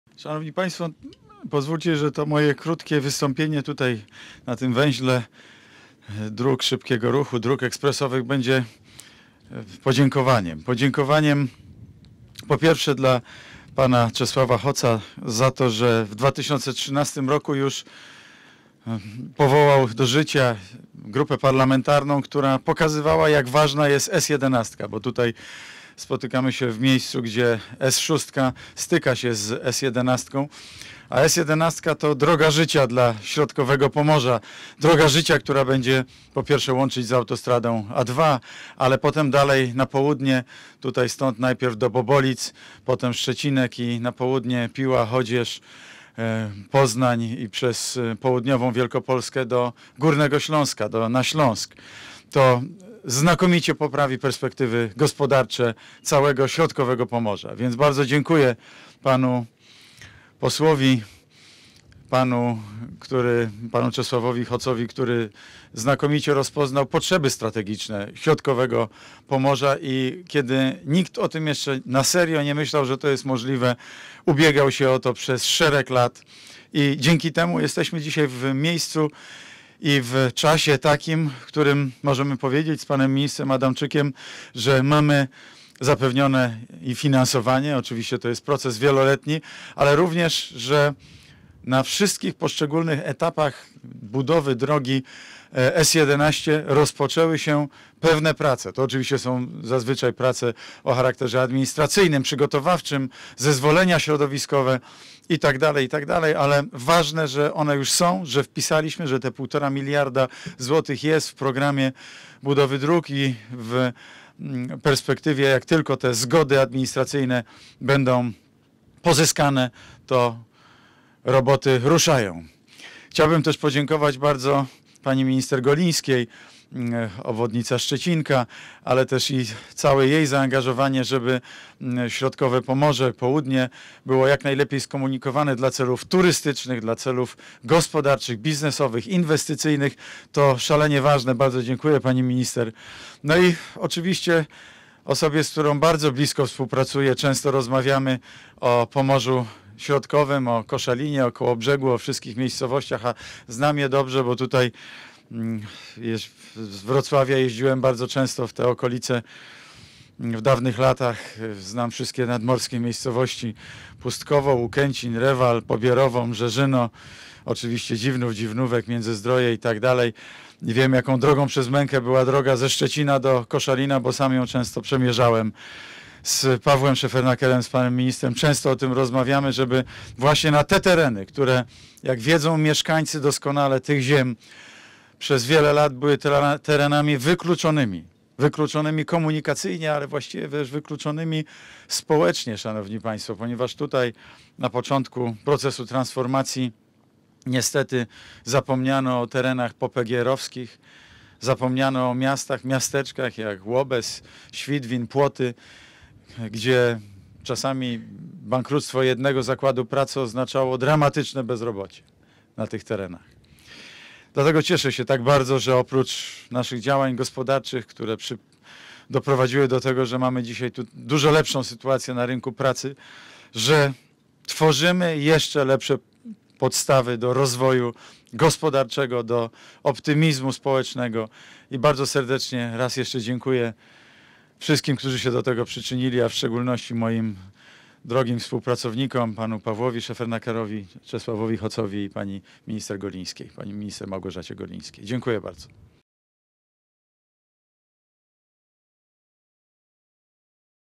Mateusz Morawiecki podczas konferencji dot. nowej drogi S6 Kołobrzeg – Koszalin
Szef rządu wziął udział w briefingu na temat realizacji odcinków dróg ekspresowych S6 i S11 w województwie zachodniopomorskim. Na ukończeniu jest odcinek drogi ekspresowej S6 łączący Kołobrzeg i Koszalin oraz część obwodnicy Koszalina i Sianowa. "Te tereny przez wiele lat były wykluczone komunikacyjne i społecznie" - mówił.